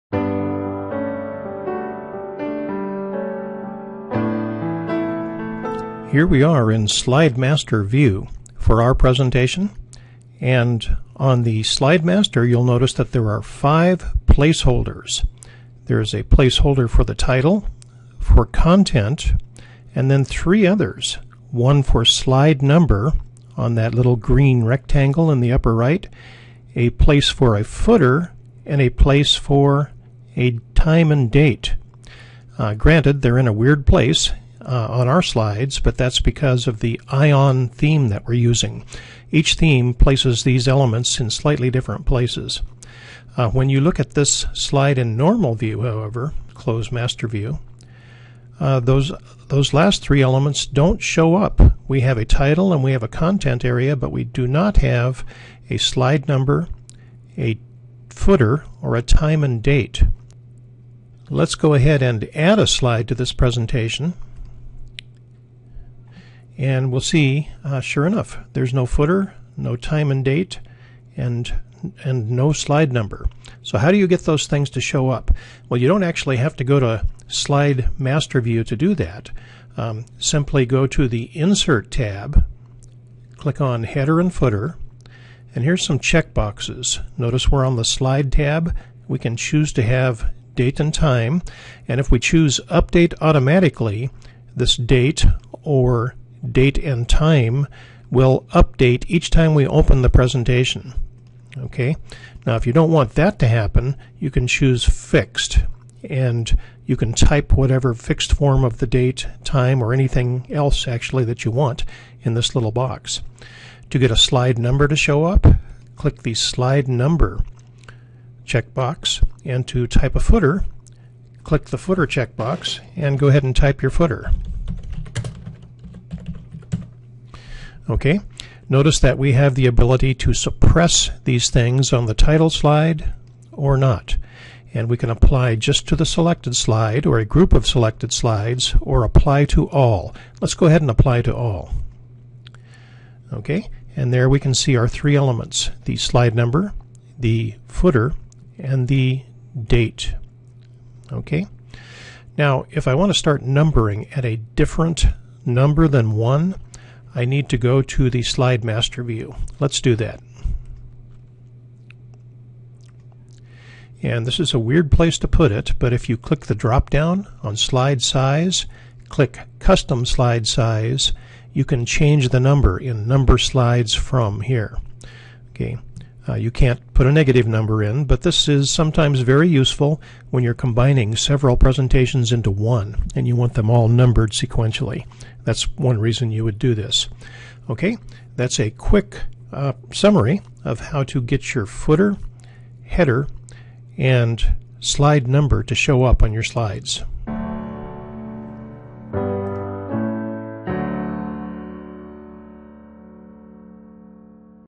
Date, Slide Number and Footer
How to get the date-time, slide number and footer to appear in a PowerPoint presentation.